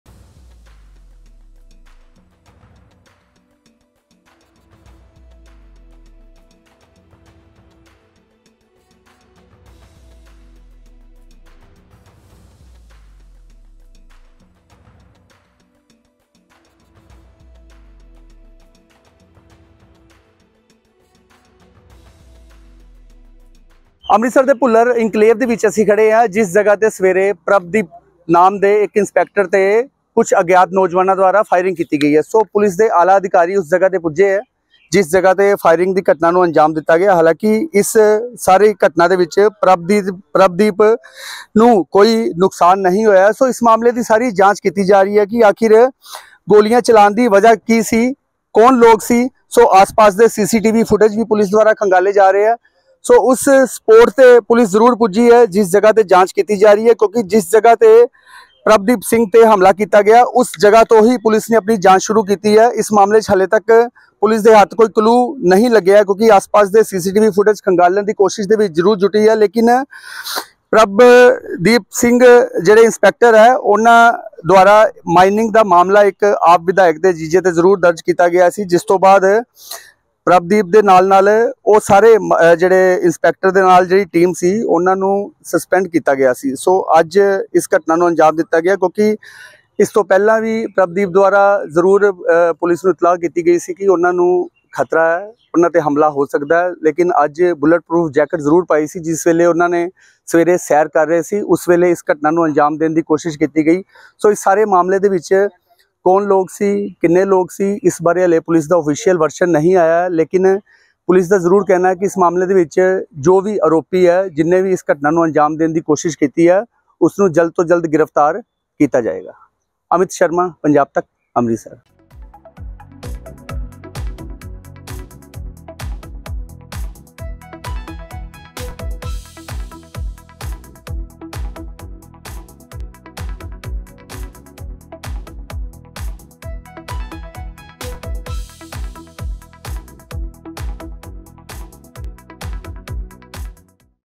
Amritsar 'ਚ ਸ਼ਰੇਆਮ CIA Inspector ਨੂੰ ਬਦਮਾਸ਼ਾਂ ਨੇ ਮਾਰੀਆਂ ਗੋਲੀਆਂ, AAP MLA ਦੇ ਜੀਜੇ 'ਤੇ ਕੀਤੀ ਸੀ ਕਾਰਵਾਈ
ਅੰਮ੍ਰਿਤਸਰ 'ਚ ਵੱਡੀ ਵਾਰਦਾਤ
CIA Inspector ਨੂੰ ਬਦਮਾਸ਼ਾਂ ਨੇ ਮਾਰੀਆਂ ਗੋਲੀਆਂ
ਅੰਮ੍ਰਿਤਸਰ ਦੇ ਫਤਿਹਗੜ੍ਹ ਚੂੜੀਆਂ ਰੋਡ ਤੇ ਮਾਰੀਆਂ ਗੋਲੀਆਂ
AAP MLA ਦੇ ਜੀਜੇ 'ਤੇ ਕੀਤੀ ਸੀ ਕਾਰਵਾਈ
ਅੰਮ੍ਰਿਤਸਰ ਤੋਂ ਅਮਿਤ ਸ਼ਰਮਾ ਦੀ ਰਿਪੋਰਟ